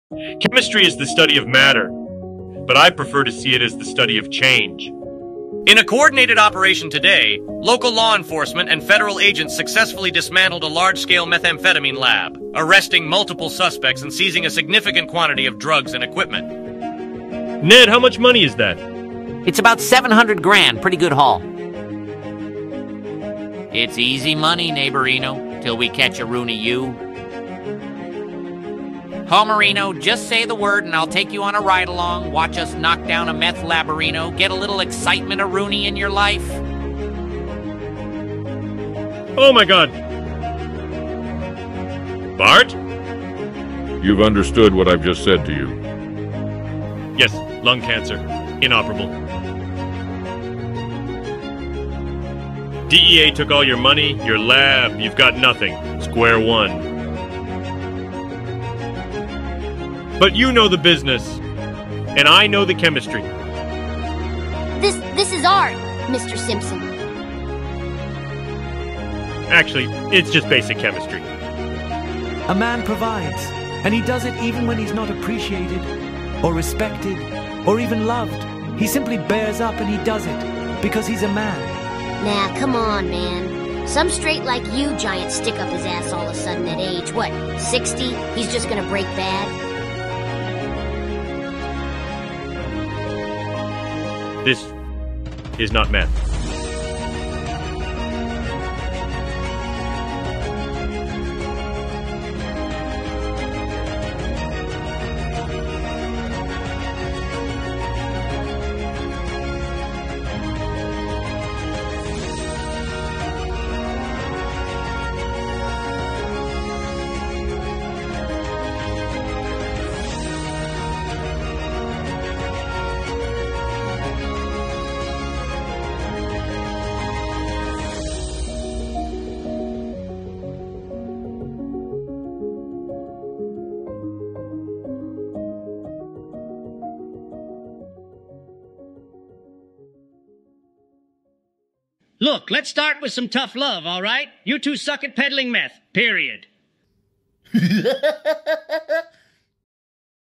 The Simpsons as Breaking Bad | Interdimensional TV
Get ready for a wild ride as we take you to a parallel universe where the iconic world of The Simpsons collides with the intense drama of Breaking Bad. In this episode of Interdimensional TV, we present Breaking Simpsons – a mashup that blends the lovable characters of Springfield with the dark, gritty reality of Albuquerque's criminal underworld.

Witness Homer Simpson transform into a real-life Heisenberg, while Marge, Bart, and the rest of the gang navigate this surreal, dangerous new environment. This groundbreaking mashup, brought to life by the power of AI and GPT-4, pushes the boundaries of what is possible with entertainment.